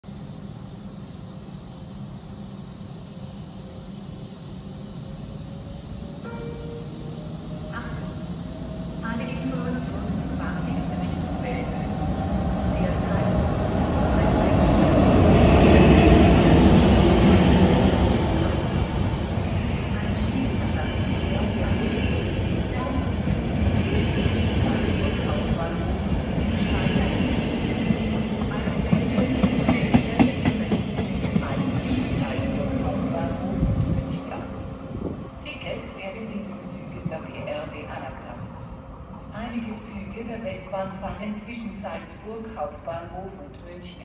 Ausfahrt ÖBB 1144 + Intercity, Bischofshofen
Eine ÖBB 1144 fährt mit einem ÖBB Intercity aus Bischofshofen aus.

An Austrian Class 1144 departs with an Intercity in Bischofshofen.